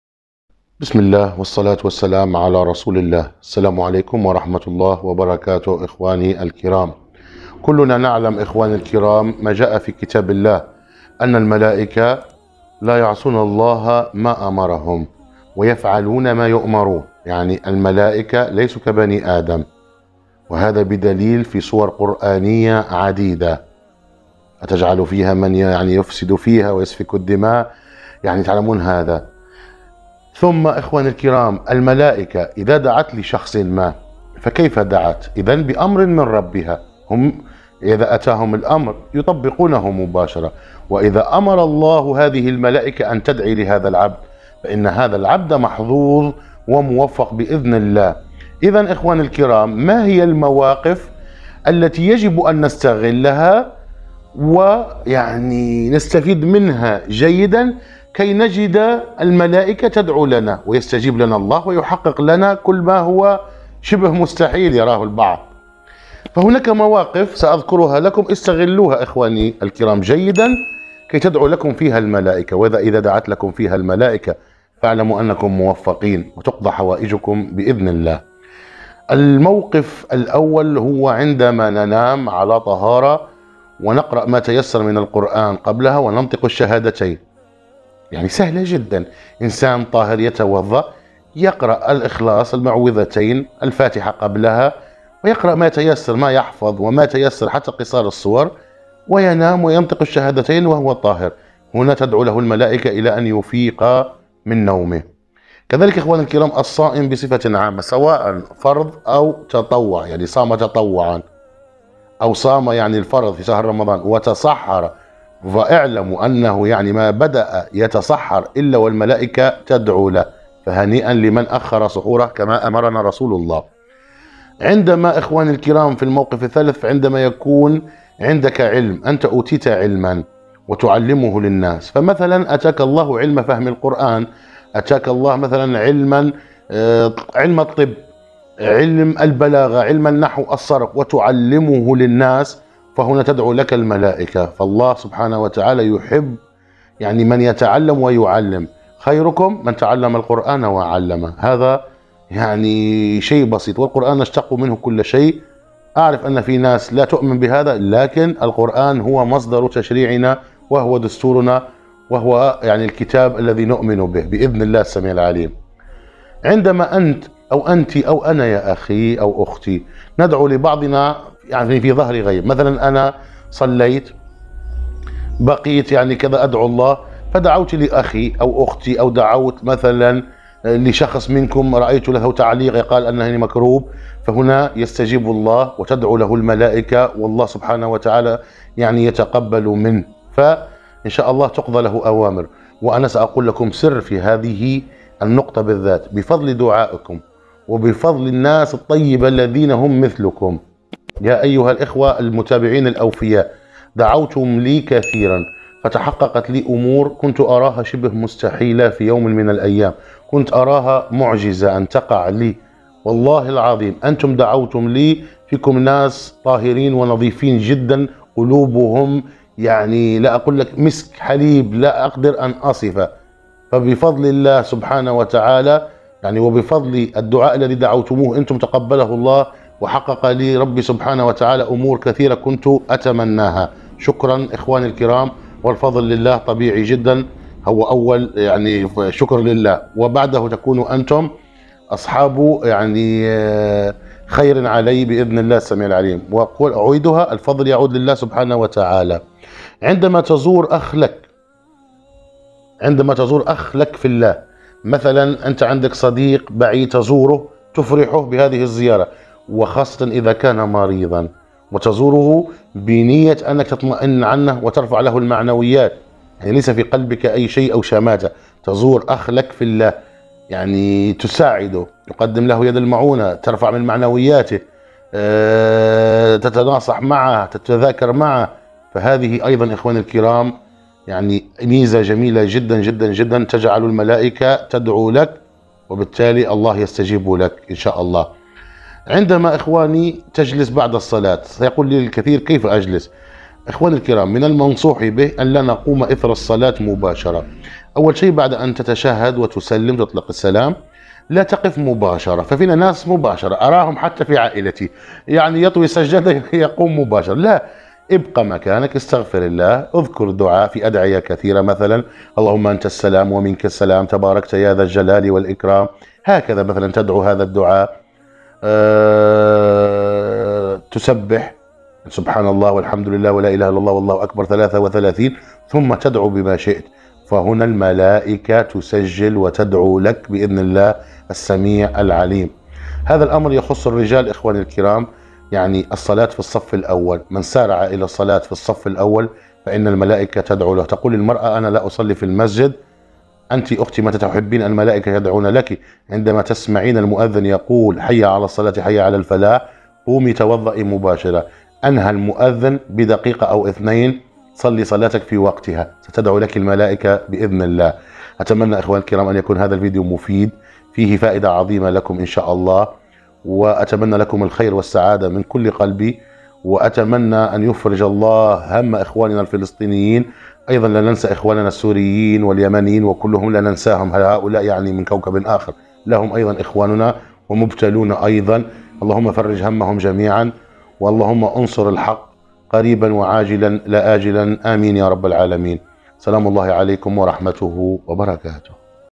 مواقف تدعي لك فيها الملائكة فلا تضيع الفرصة
إليك ثمانية مواقف مهمة في حياتك تدعوا لك فيها الملائكة مباشرة باذن الله تعالى
تفسير الاحلام _شاكر الكرامتي #